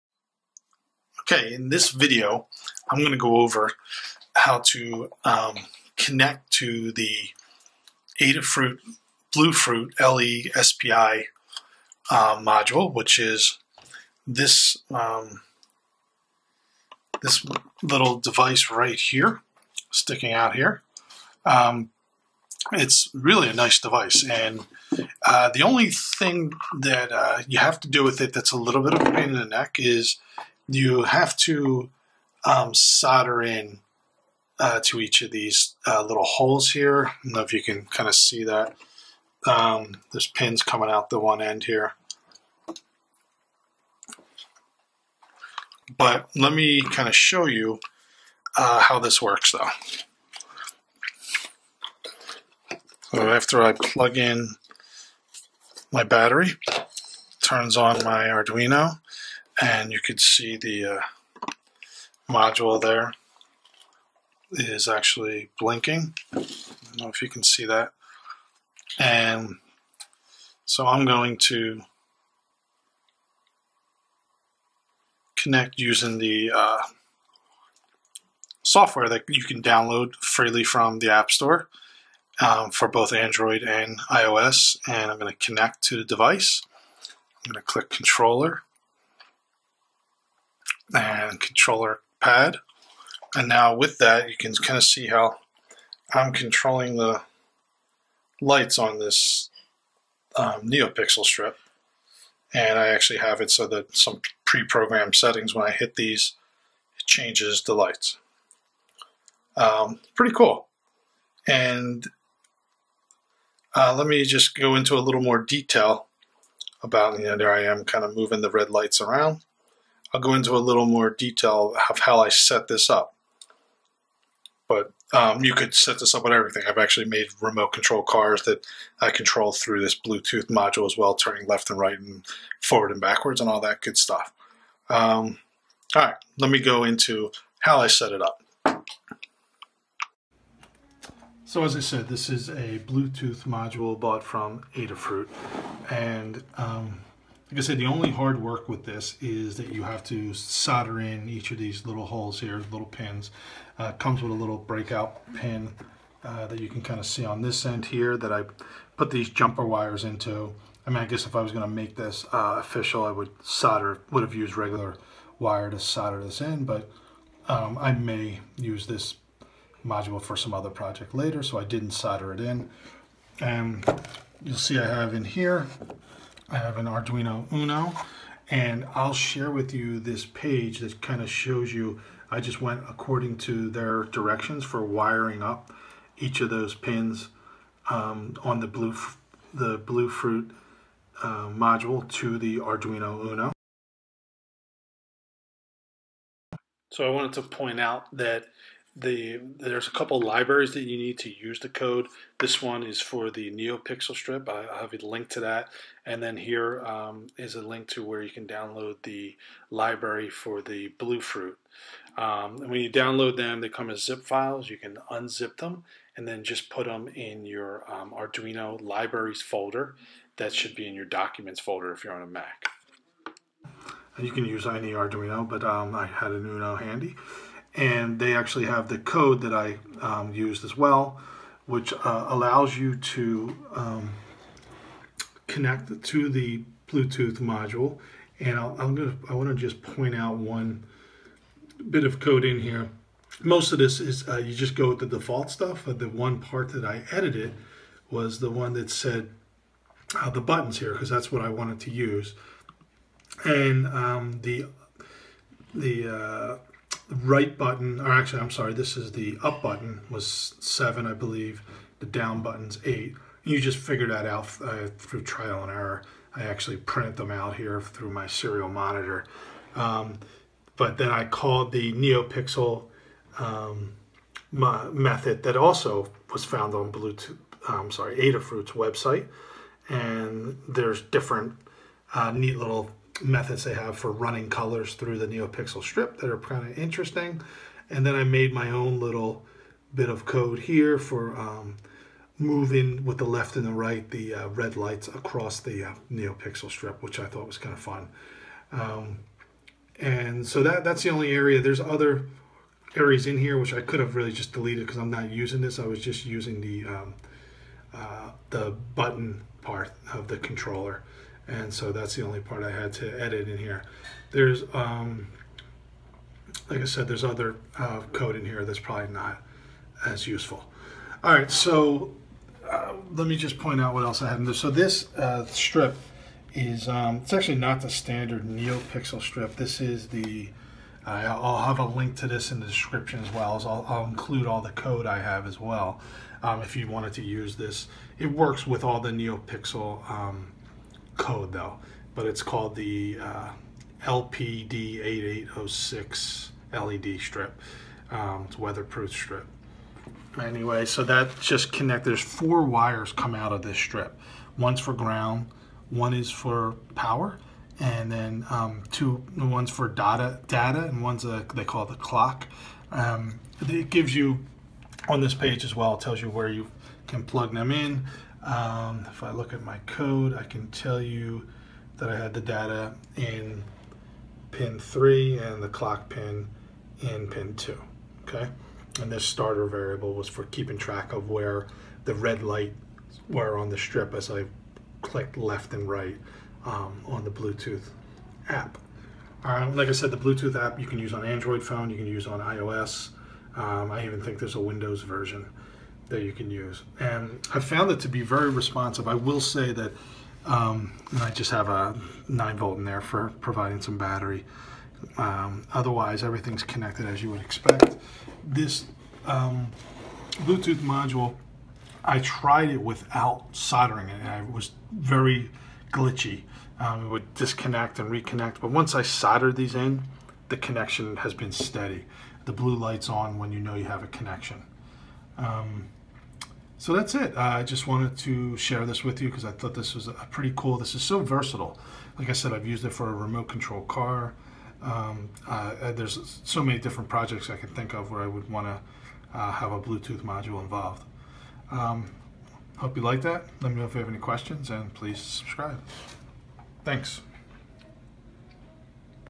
Bluetooth Controlled Lights with Arduino
This video is explains how to use Adafruit's BlueFruit Bluetooth controller to control NEOpixels.